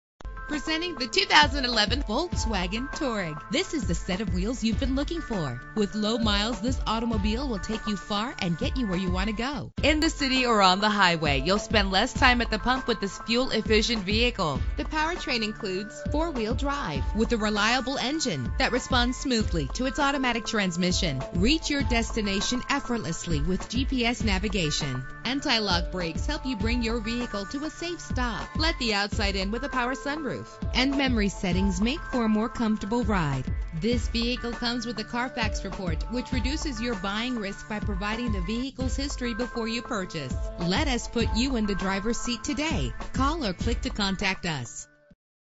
2011 Volkswagen Touareg - Raynham MA
This vehicle represents similar vehicles available at Silko Honda.
Contact Silko Honda at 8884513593, for availability of specific vehicles.
Year: 2011
Make: Volkswagen
Model: Touareg
Trim: TDI Lux
Engine: 3.0 liter 6 cylinder 24 valve DDI DOHC Turbo Diesel
Transmission: 8-Speed Automatic
Color: White
Mileage: 7611
Address:
1580 New State Hwy
Rt. 44